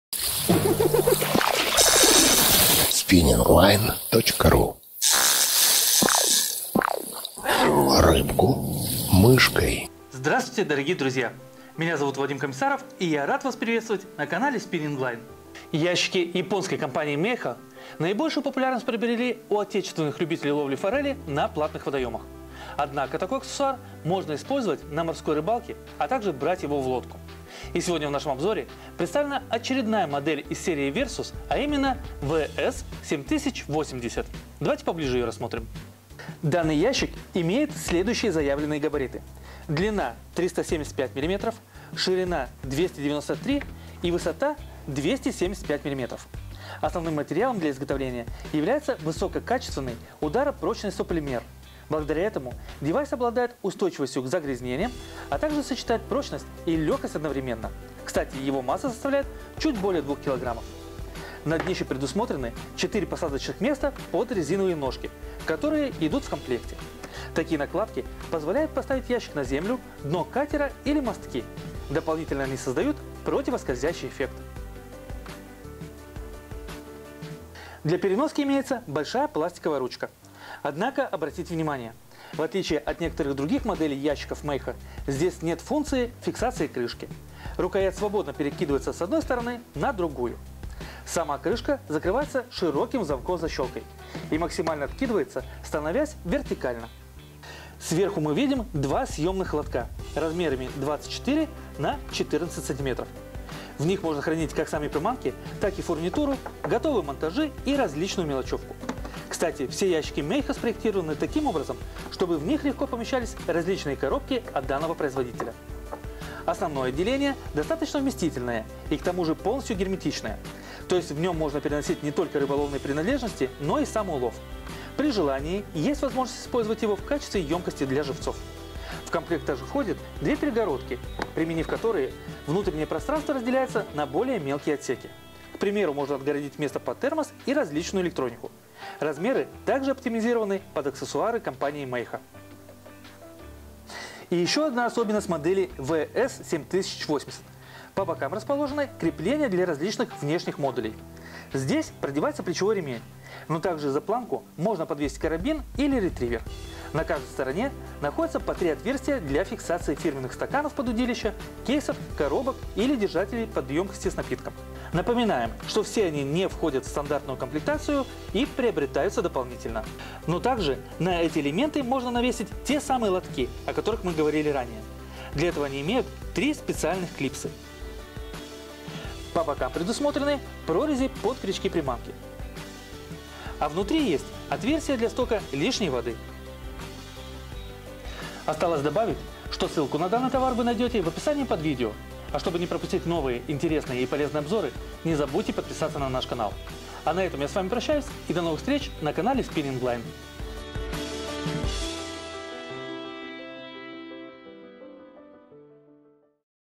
Ящик Meiho Versus VS-7080
Для комфортного хранения всех мелочей оснащения и инструментов фирма Мейхо предлагает модель ящика Versus VS-7080. Особенно такие ящики Meiho полюбились тем рыбакам, которые предпочитают ловить форель на платниках. В лодке этот компактный ящик также будет очень удобен, так как в комплекте идут резиновые ножки для лучшей устойчивости. Сделан из ударопрочного сополимера, который обеспечивает не только надежность изделия, но и небольшой вес. Внутреннее отделение очень вместительное и герметичное, поэтому в нем можно хранить не только снасти и инструменты, но и перевозить улов, а также использовать как емкость для живца. Рассмотреть Versus VS-7080 более подробно вы сможете в этом видео.